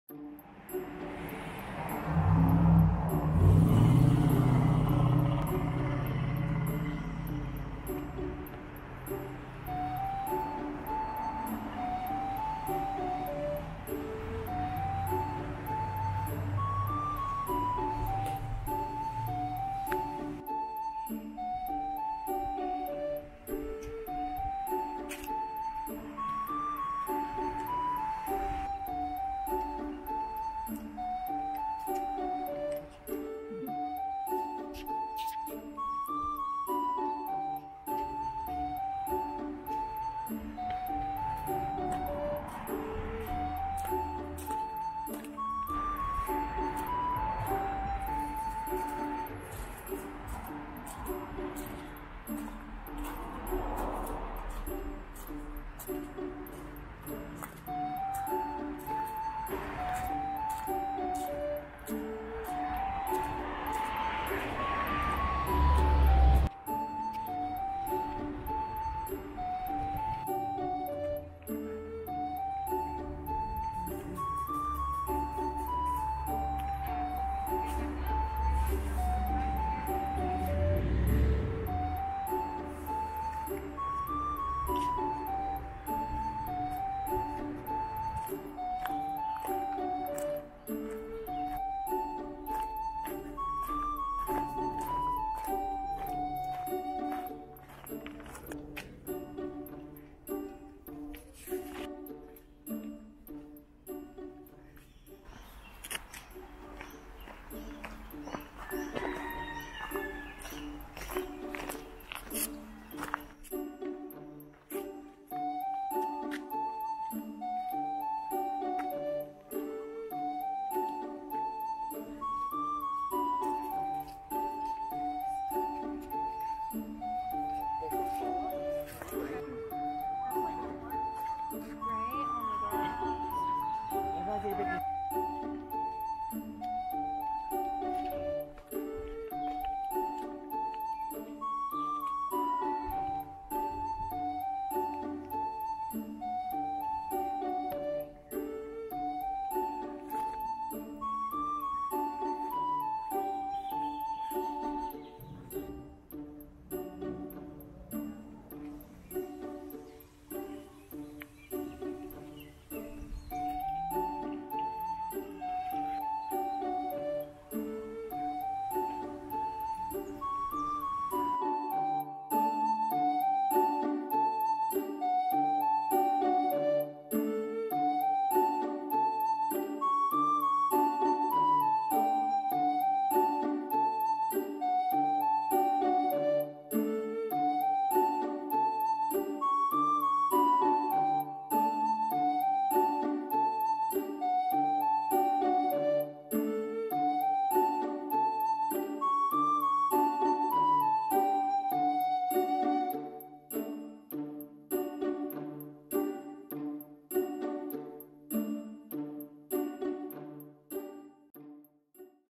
Hawaii Wahiawa Botanical Garden ハワイ☆ワヒアワ植物園にお出かけ🎵
I’ve been to Wahiawa Botanical Garden.  It is located in a middle of housing areas and it is like a park to me, but there are many plants.  It’s a good place to relax and take a break.
オアフ島 ワヒアワの植物園に行って来ました。
住宅地の中にあり公園の様な感じでしたが、緑が沢山でほっと一息、のんびりするのに良い場所です🎵

It was cloudy and not a good day for shooting, but I hope you enjoy it.
お天気が曇りで撮影日和ではありませんでしたが、楽しんで頂けたら嬉しいです。

There are more botanical gardens in Oahu. I’ll go check them another time.
オアフ島には他にもいくつか植物園があるので、また別の所にも行ってみたいと思います。
お楽しみに!

Mika